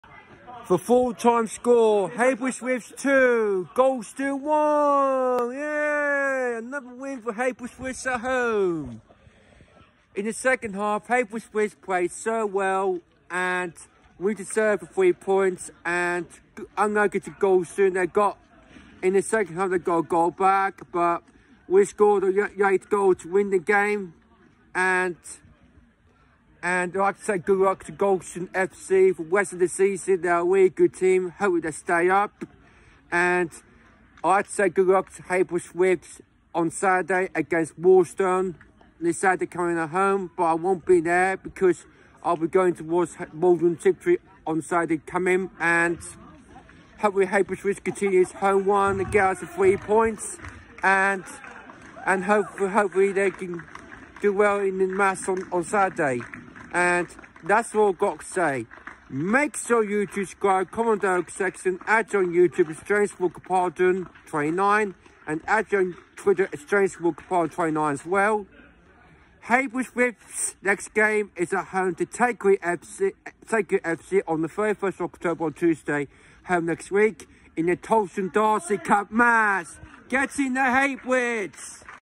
Heybridge Swifts Vs Gorleston Fc Isthmian league north Division my full time thoughts we won again
My thoughts about Heybridge swifts second half performance and they got a another 3 points at home and my support thought out the whole match was fantastic again and the players came over to say thank you for your support and also good luck to Heybridge swifts this Saturday at home to Walthamstow Fc becauseI won’t be there because I will be watch Maldon Tiptree this Saturday at home to Stowmarket town Fc because they had a waterlogged pitch in Stowmarket but I be back with Heybridge swifts on the 31 of October well we play Takeley Fc in the Tolleshunt d arcy cup game at home up the Swifts United United